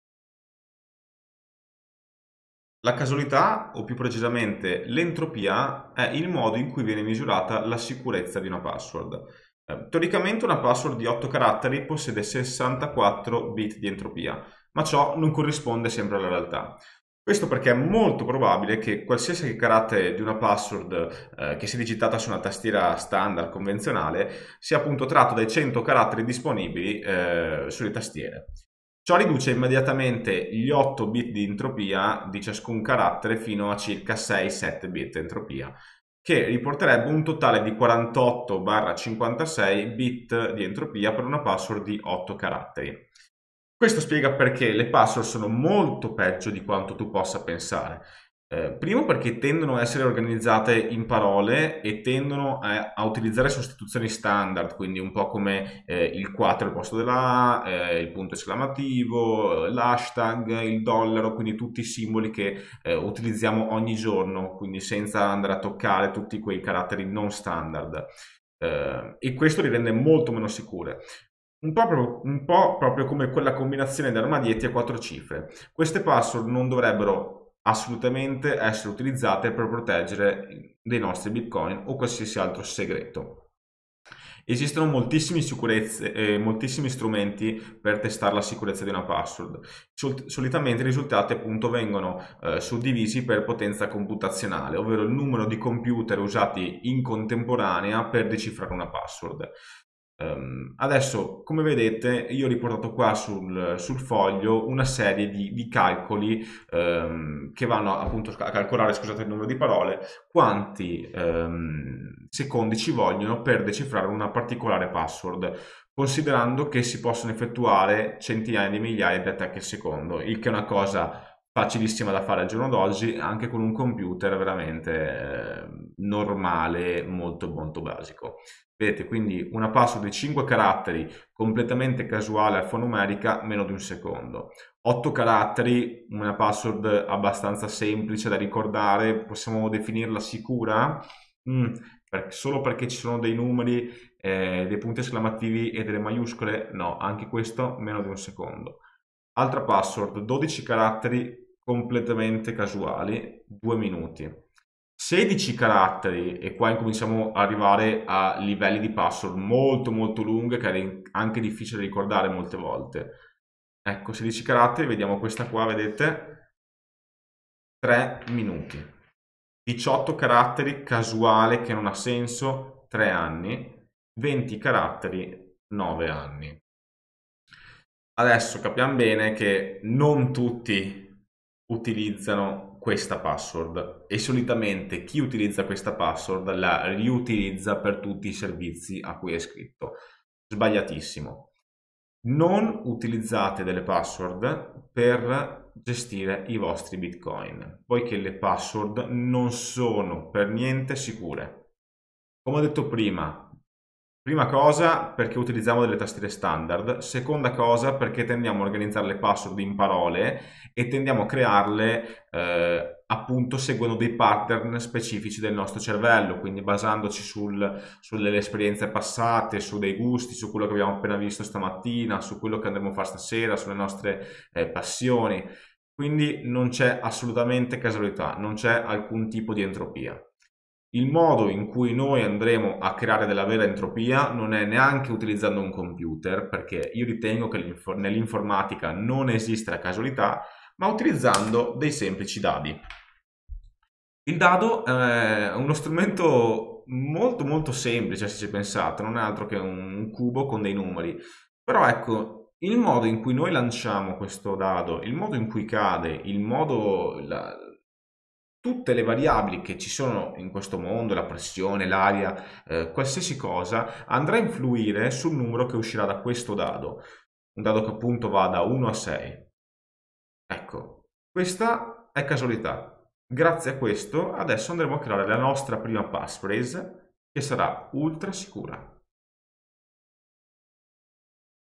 BitcoinAlSicuro 02 - P4ssw0rd
Non c'è abbastanza casualità in una password per essere un segreto indecifrabile a meno che non sia molto lunga e fondamentalmente impossibile da memorizzare.